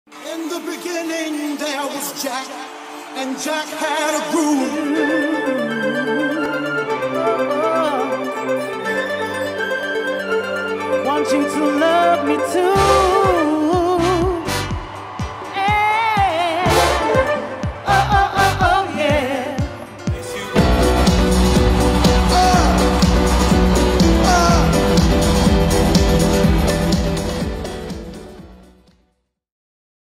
Hacienda Classical - Manchester Castlefield Bowl 2023
As FAC51 The Hacienda rounds off its 40th anniversary year with its fully sold out Depot Mayfield gig and the BBC2 broadcast of “The Hacienda : The Club That Shook Britain”, attention turns to 2023 and in our first event announced for next year, Hacienda Classical returns for a record breaking appearance at Castlefield Bowl Saturday 8th July 2023.

Marking its seventh Castlefield event at “Sounds Of The City” in the eight years since 2016, the annual homecoming is one of the city’s events of the year as The Hacienda’s crowd, DJ Graeme Park, Manchester Camerata and special guests come together for what is now becoming a legendary party.